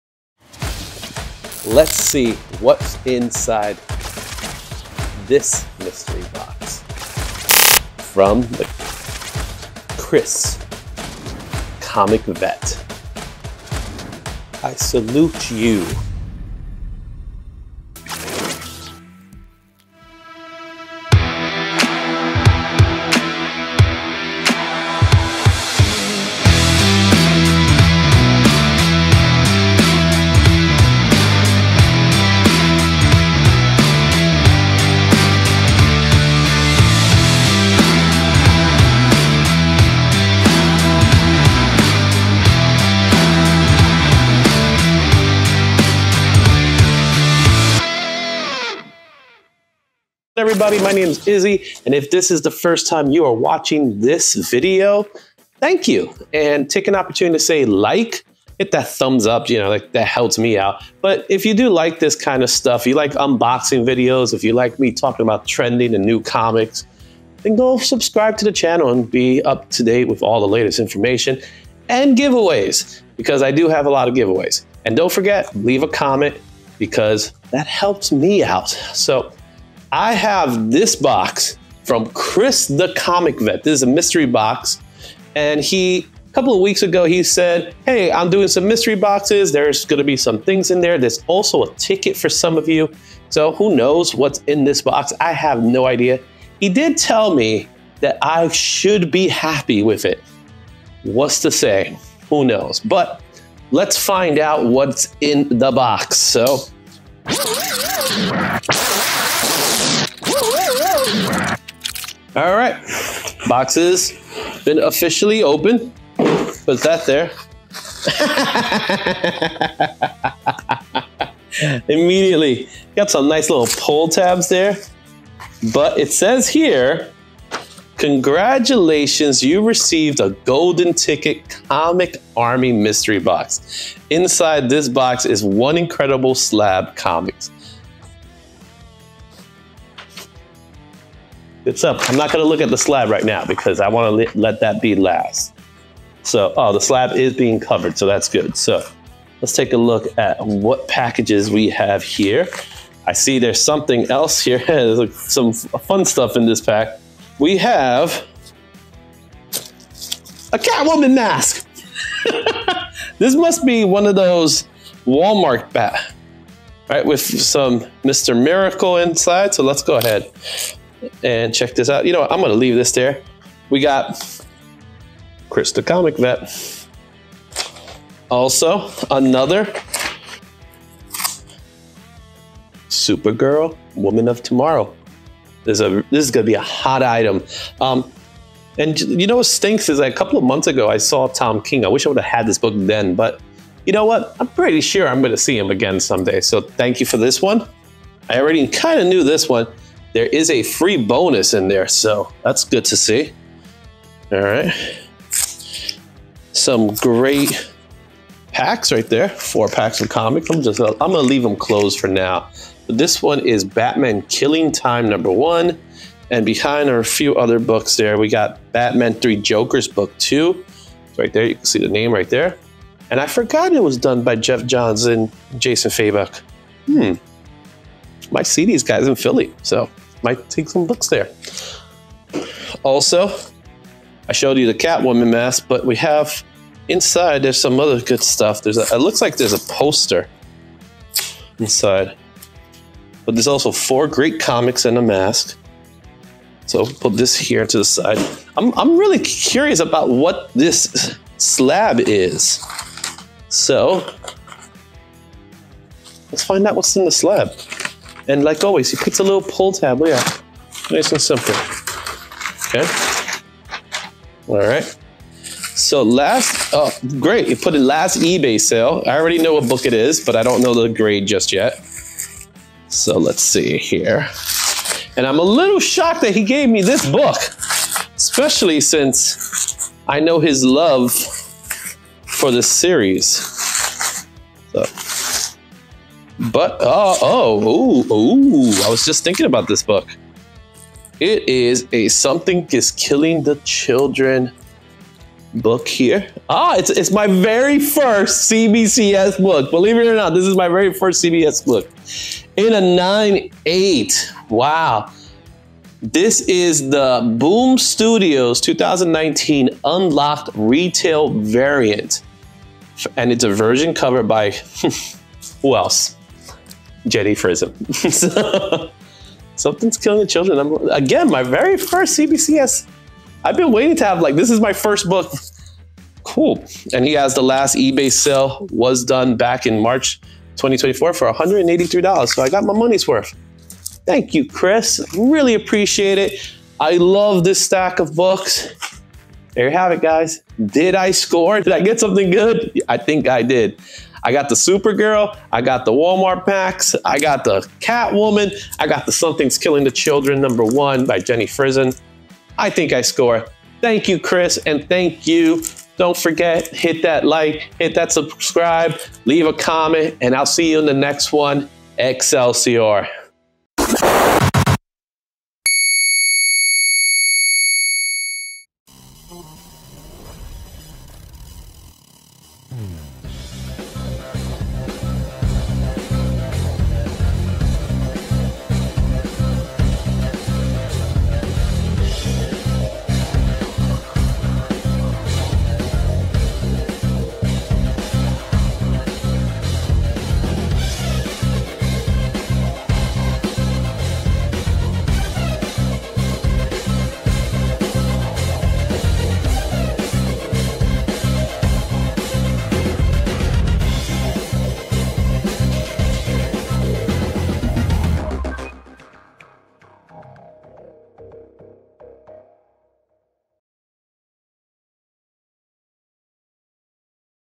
Unboxing a $100 Mystery Comic Box from @thecomicvet What Surprises Await?
Join me as I unbox a $100 mystery comic box from Chris the Comic Vet! See what hidden gems and rare finds I uncover in this exciting unboxing video.